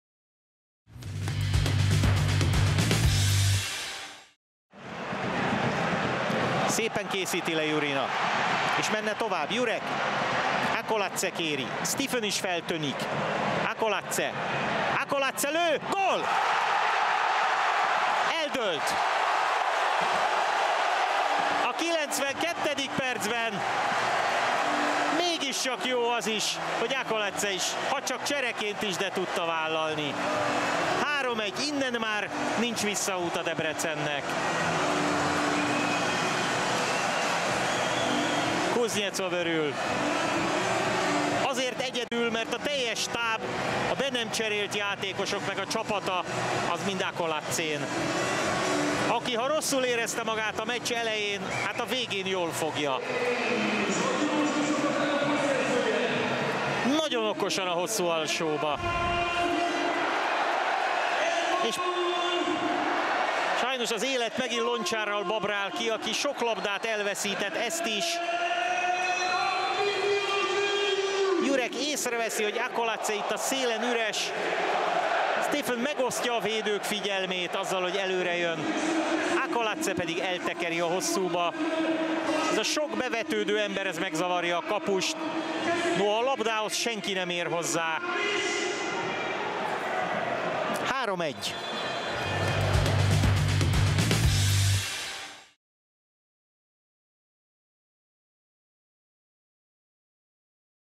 Acolatse Elton-Ofoi gólja a DVTK – DVSC mérkőzésen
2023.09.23. OTP Bank Liga, 7. forduló
DVTK – DVSC | 3-1 | (2-0) |
Acolatse Elton-Ofoi gólja